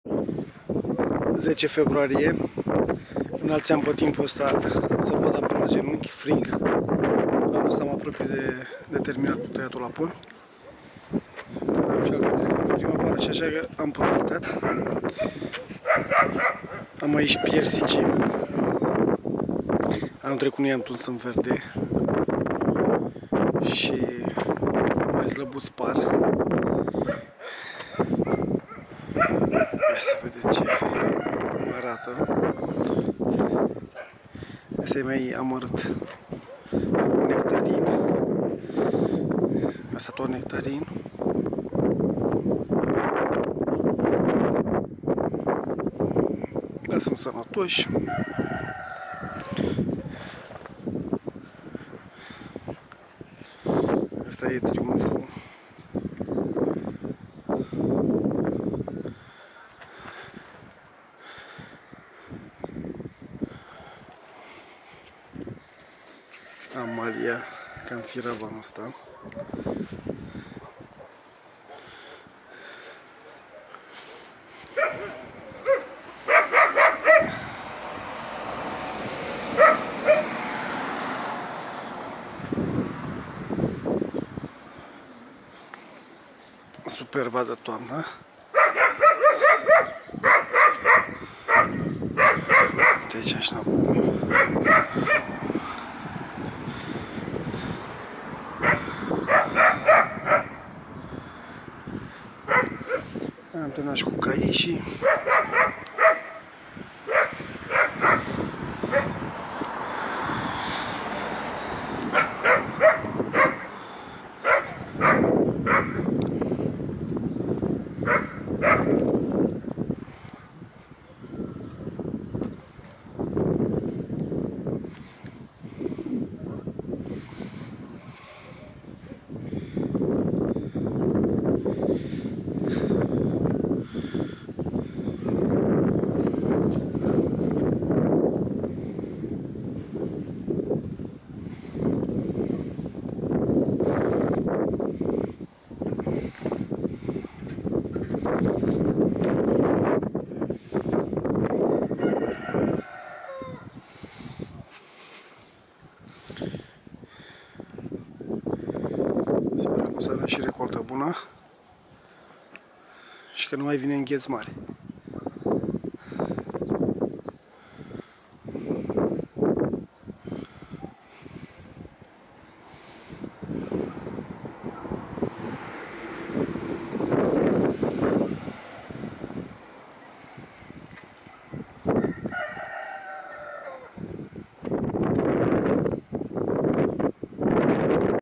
Piersici si nectarini după tăiere 10.02.2016
Am profitat de vremea buna si terminat de tuns pomii din mica mea livada.